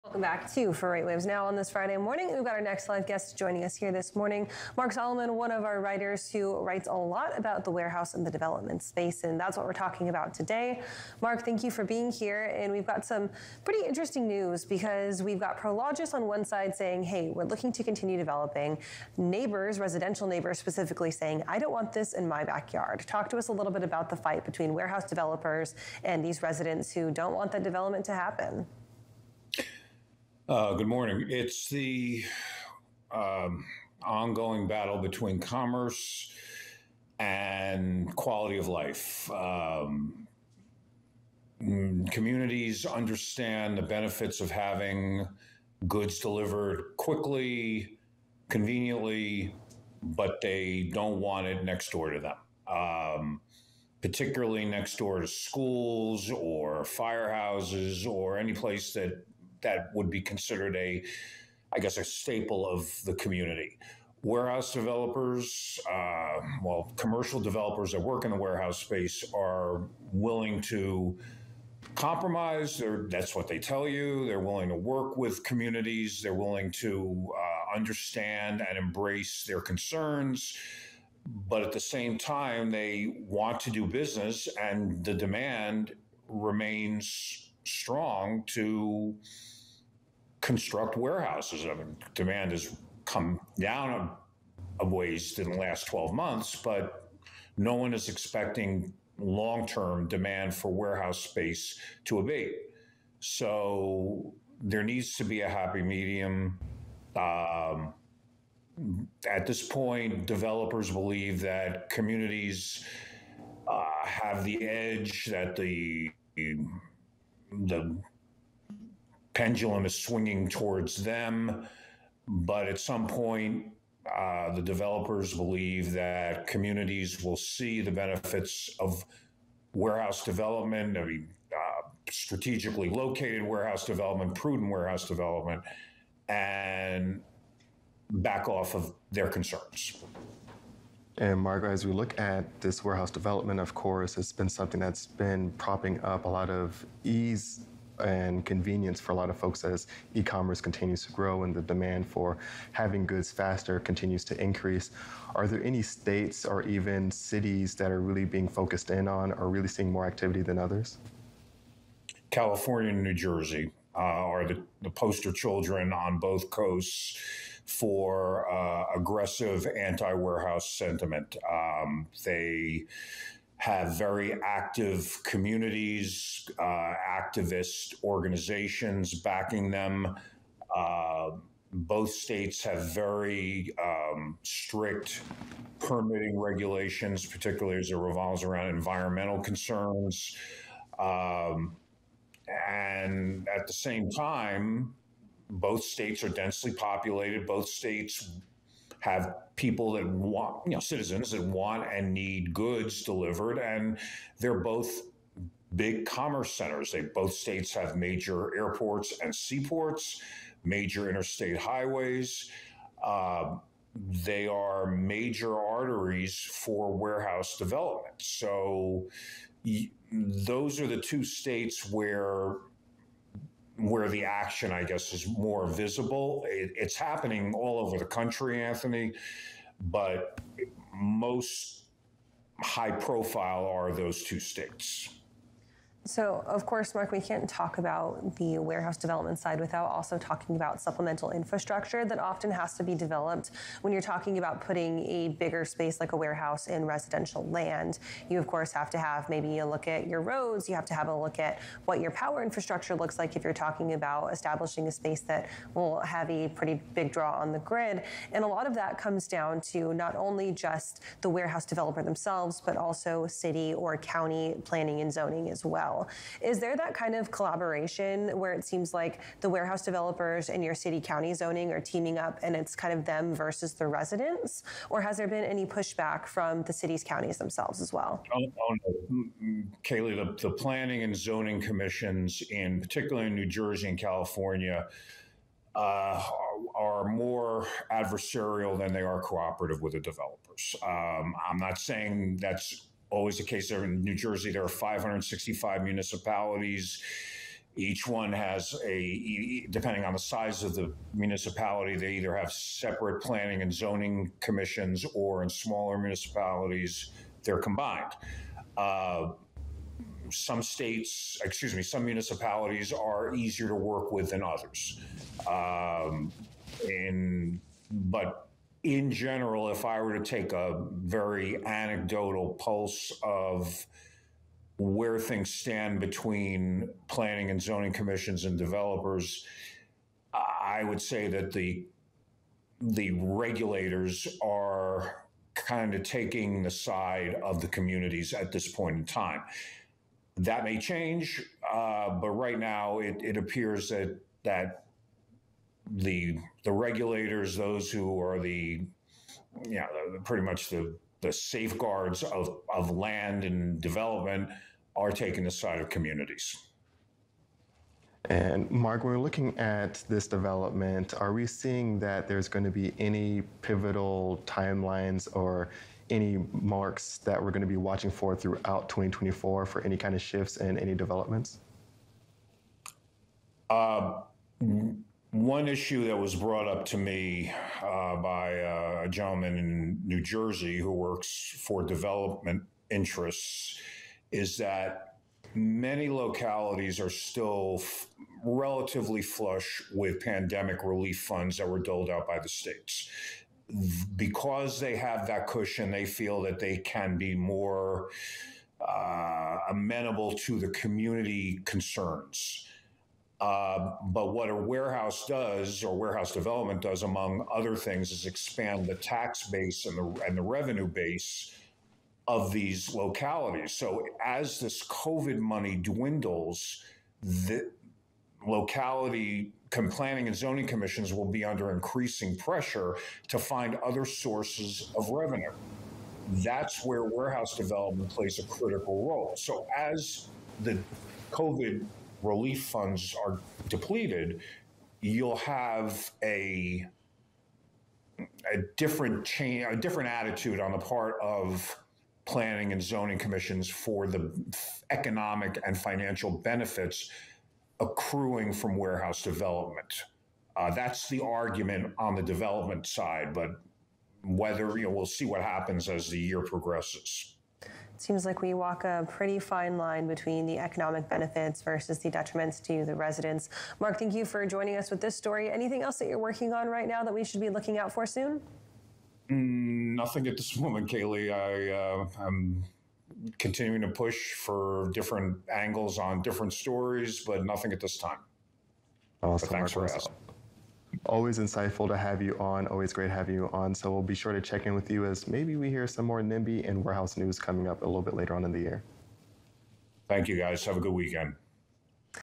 Warehousing developers face pushback in residential areas
In the second hour, Mark Solomon reports on the difficulty of warehousing development and why more people are asking developers not to put warehouses in their backyards.